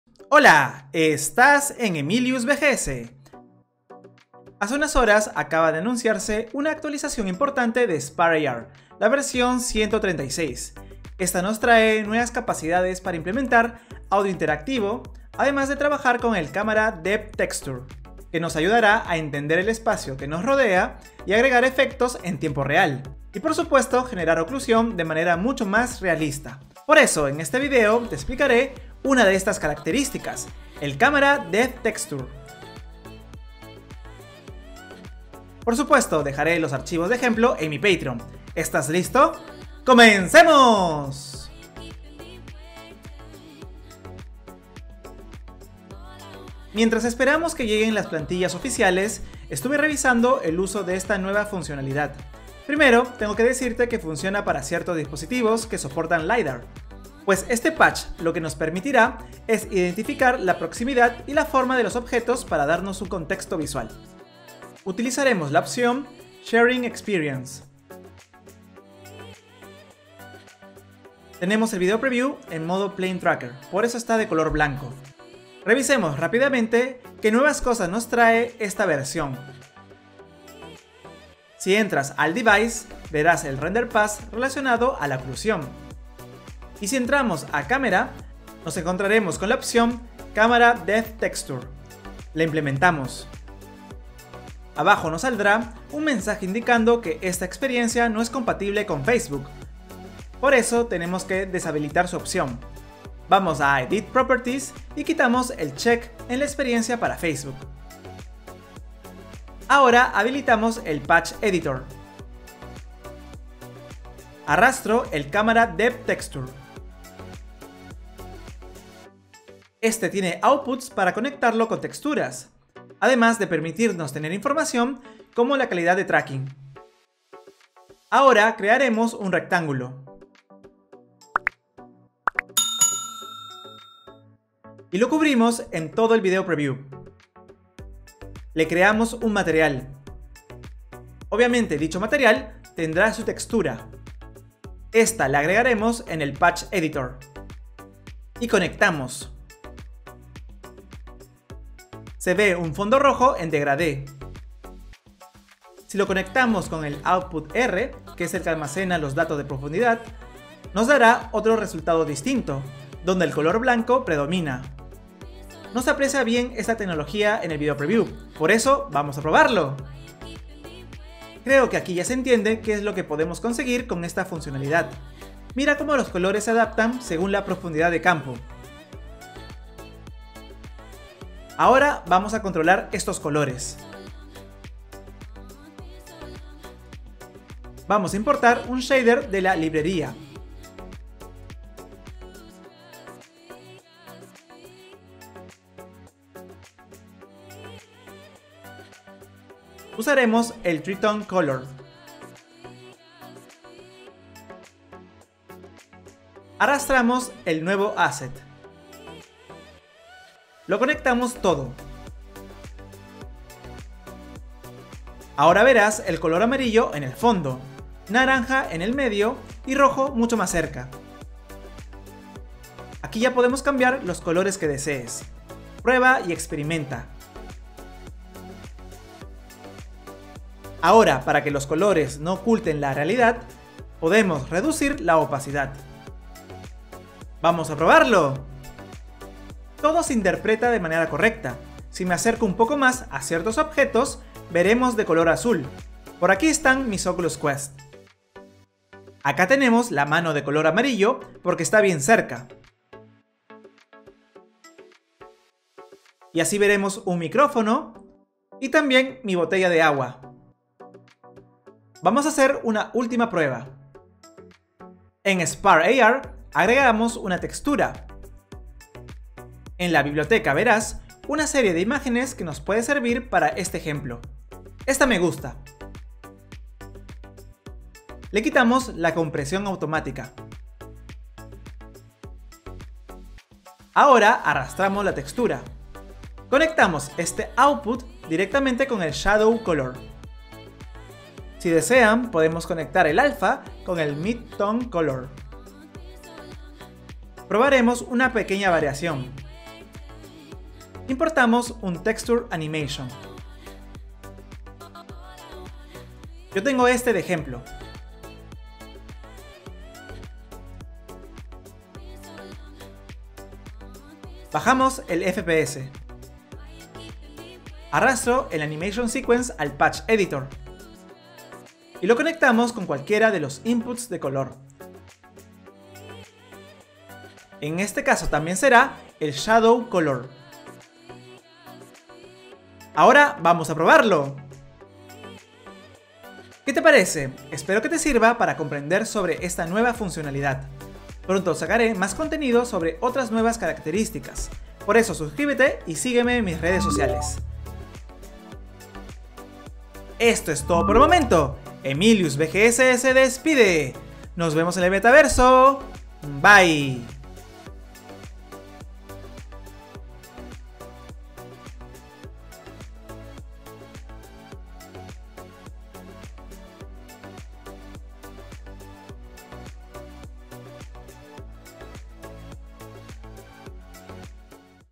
Usando Camera Depth Texture - Spark AR New feature!!! (LiDAR) (EN Subtitles)
La nueva versión de Spark AR V136 nos trae interesantes funcionalidades para interactuar con el sonido, detectar la profundidad de la escena e incorporar oclusión de manera precisa. En este video te explicaré como funciona el Camera Depth Texture.
The new version of Spark AR V136 brings us interesting features to interact with sound, detect the depth of the scene and incorporate occlusion accurately. In this video I will explain how Camera Depth Texture works.

Mi VR / AR software y hardware:
⚡ Oculus Quest
⚡ Aryzon
⚡ Zapbox
⚡ Merge Cube